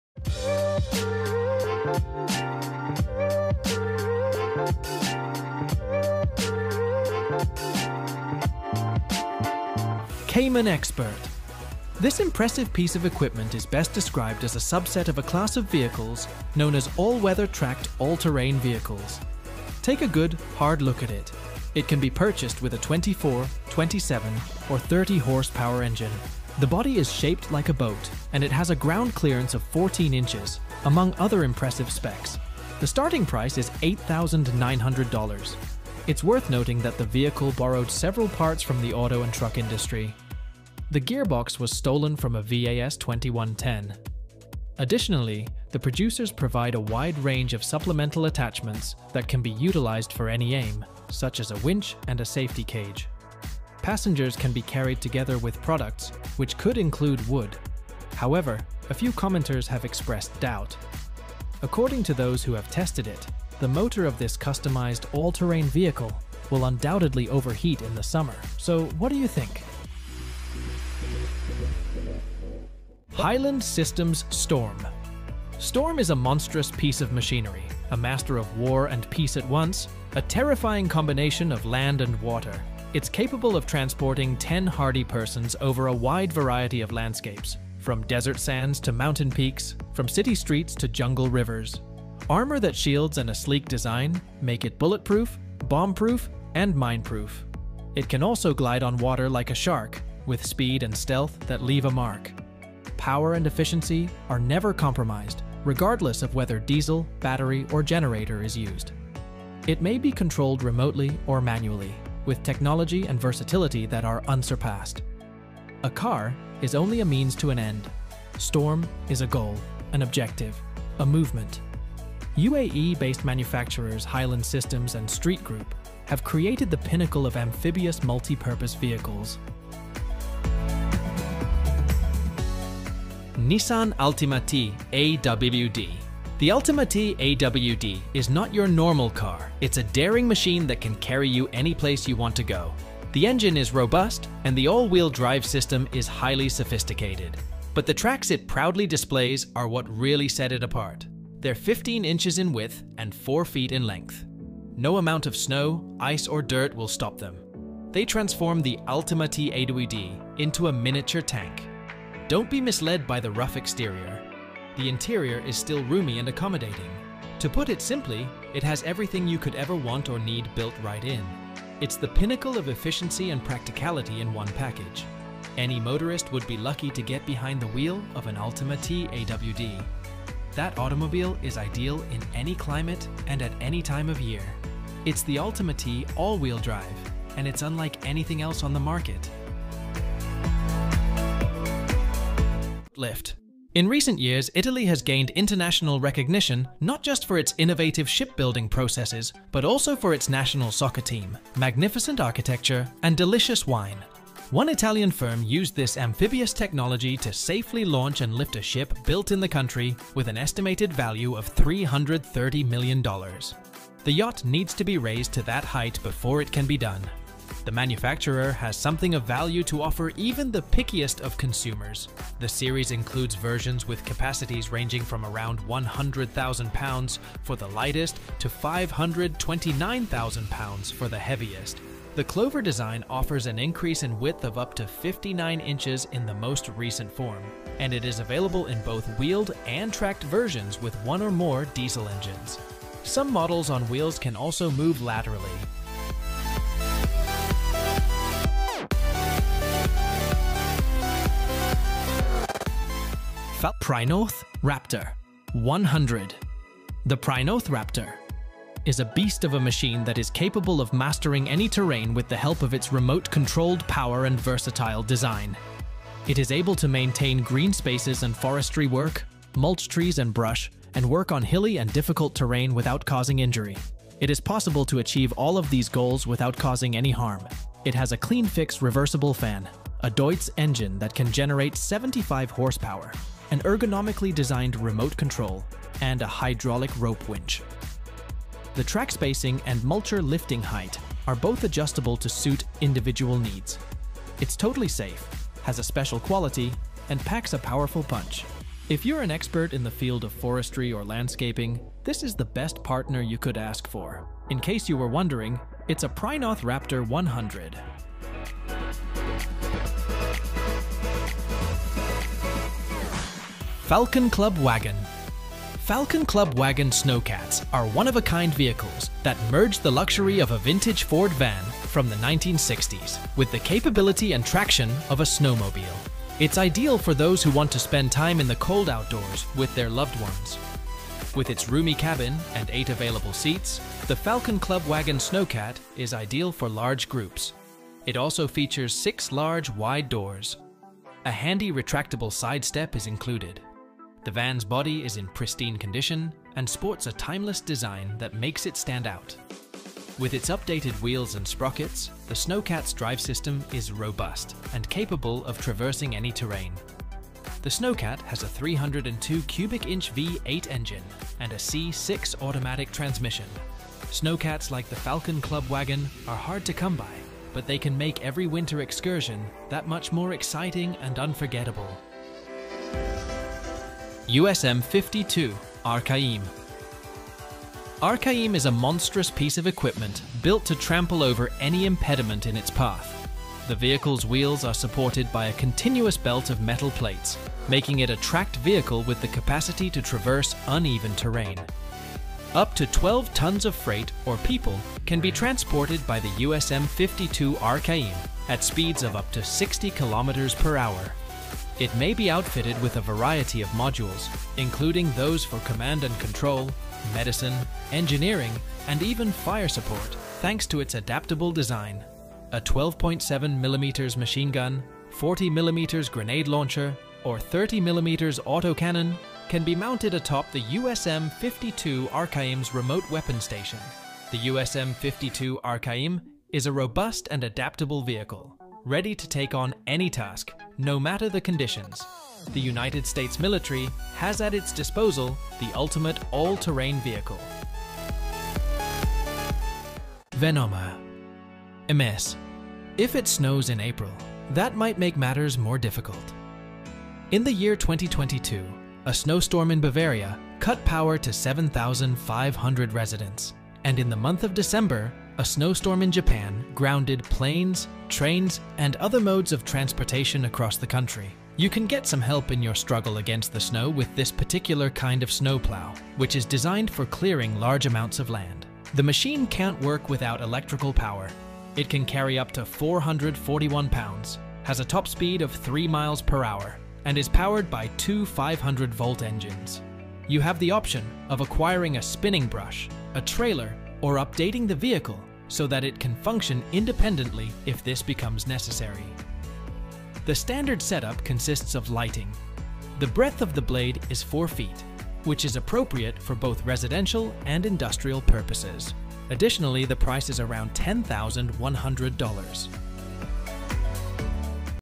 Stepping into the Future: Cutting-Edge Vehicles Set to Transform the World
Nissan Altima-te AWD

Highland Systems STORM

VENOM MS

Falcon Club Wagon

Boat Lift

CAIMAN EXPERT

PRINOTH RAPTOR 100

USM 52 arkhaim